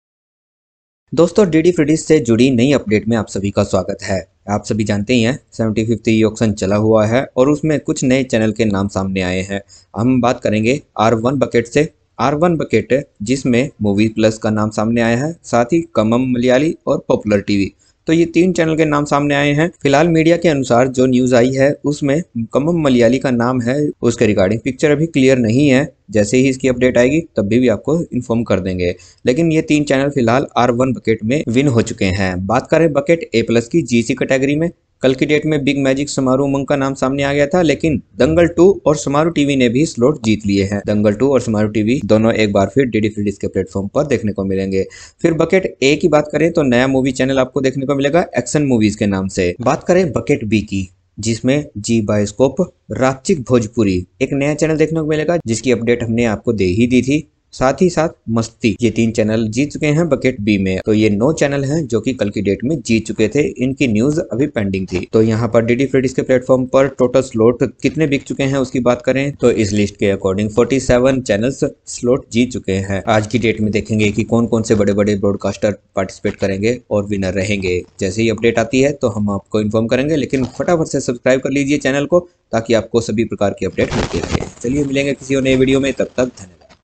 9 More Slots Sold Out In DD Free Dish Mpeg2 75 E Auction || Free Dish 75 E Auction Result
9 More Slots Sold Out In DD Free Dish Mpeg2 75 E Auction || Free Dish 75 E Auction Result